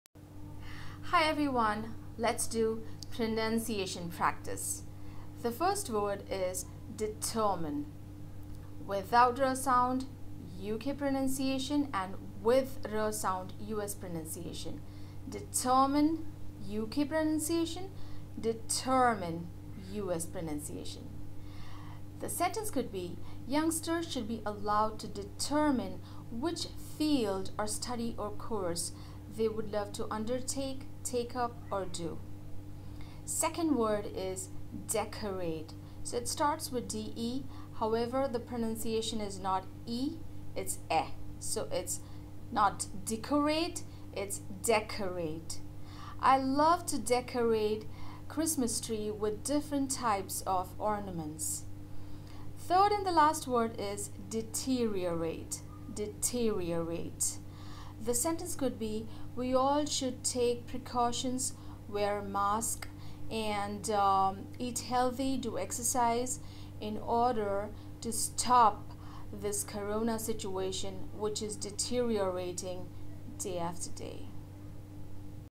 Pronunciation of English words.
How to pronounce these 3 words they are
1. Determine
2. Decorate
3  Deteriorate

While you are learning to communicate in English along with learning grammar and fluency you should also focus on the pronunciation aspect.

Learn Voice and Accent. Reduce FLI that is first language influence.

watch this video and learn the pronunciation of the words mentioned above.

I am sure this video will help you in communicating with correct pronunciation.